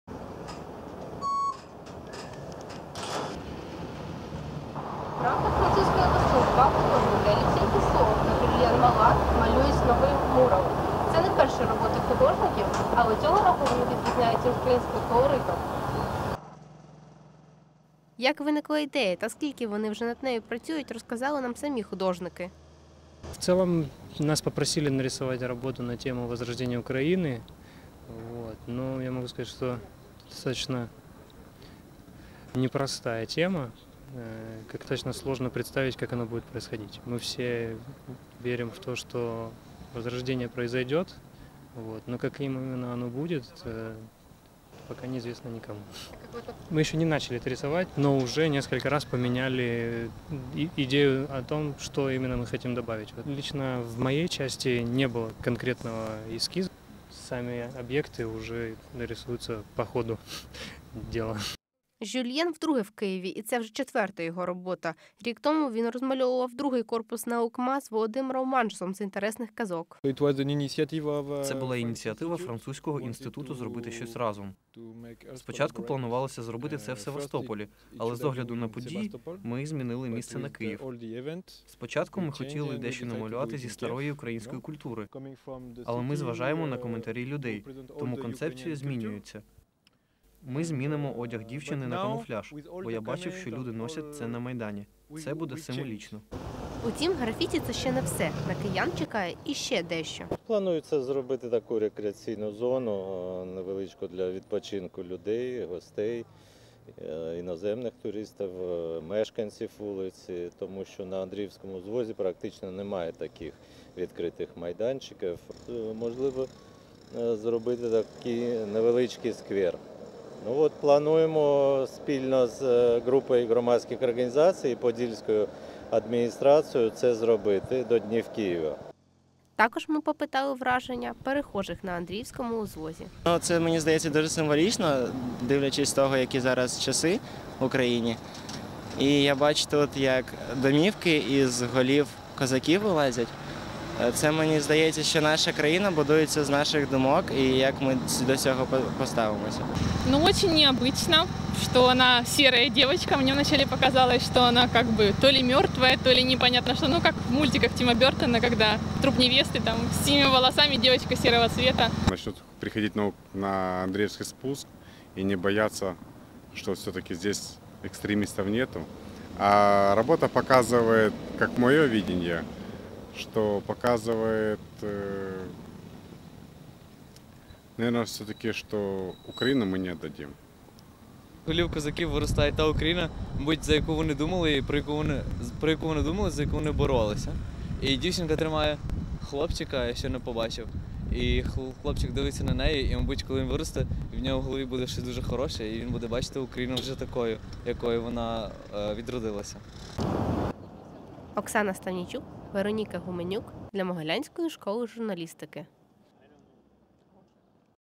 Нове графіті на Андріївському
У рамках Французької весни на Андріївському узвозі з'явилося нове графіті. Які смисли воно за собою приховує та які враження киян - у сюжеті.

Автори: Оксана СТАВНІЙЧУК,
Вероніка ГУМЕНЮК
"Епізоди", (с) Могилянська школа журналістики, 2014